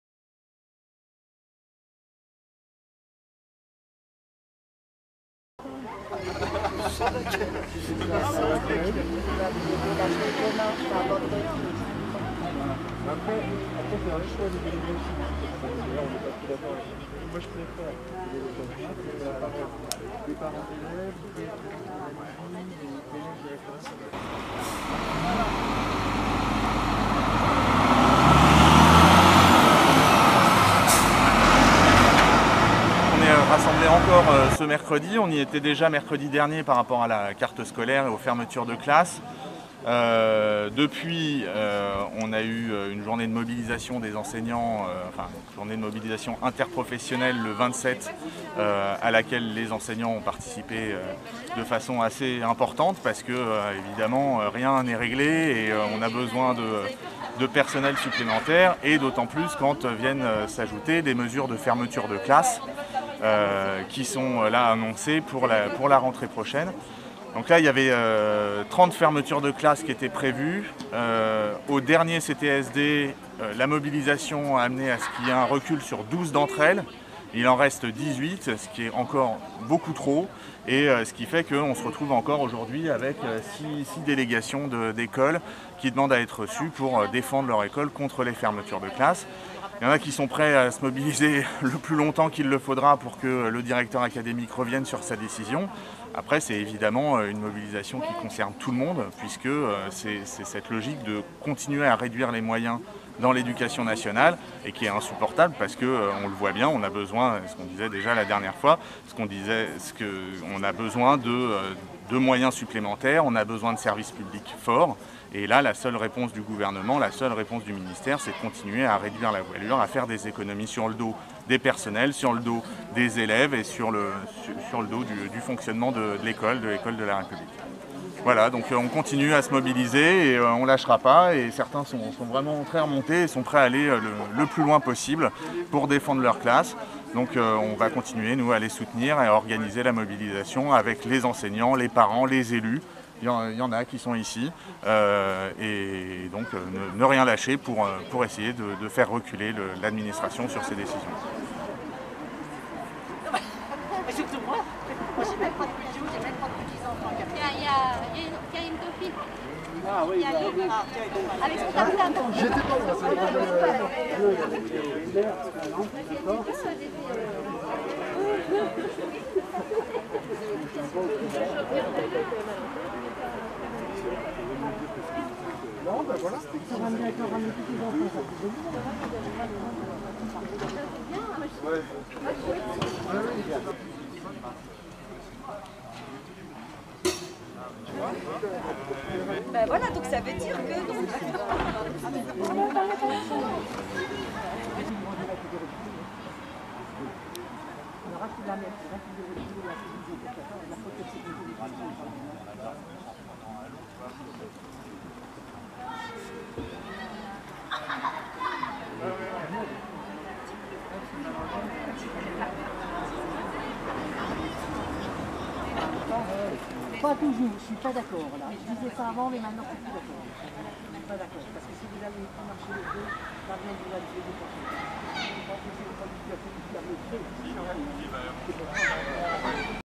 Convergence - Rassemblement contre les suppressions de poste dans l’Éducation Nationale - 02-02-2022
Convergence - Rassemblement contre les suppressions de poste dans l’Éducation Nationale - Auxerre, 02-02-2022